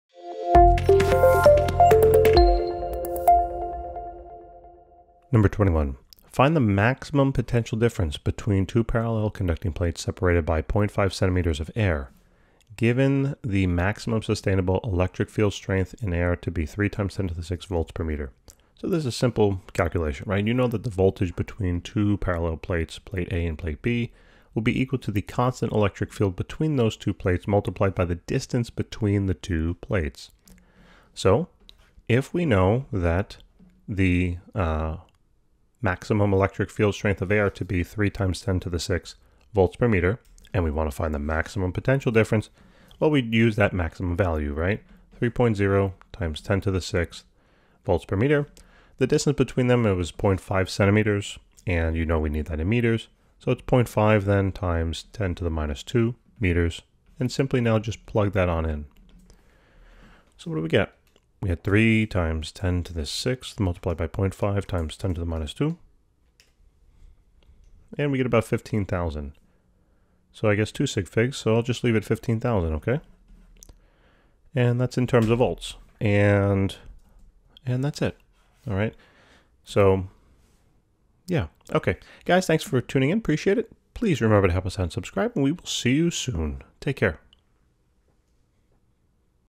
19.21 | Find the maximum potential difference between two parallel conducting plates separated by
Find the maximum potential difference between two parallel conducting plates separated by 0.500 cm of air, given the maximum sustainable electric field strength in air to be 3.0×10^6 V/m.